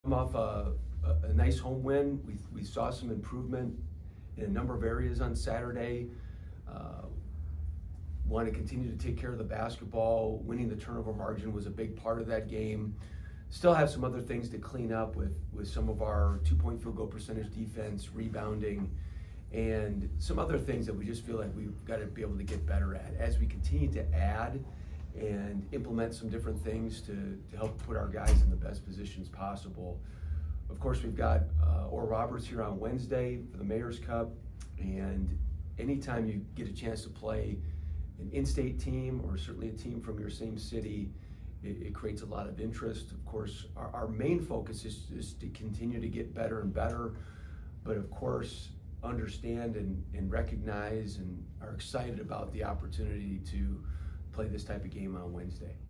Tulsa coach Eric Konkol previews Mayor's Cup
Tulsa men’s basketball coach Eric Konkol meets the media and previews his team’s game against Oral Roberts on Wednesday.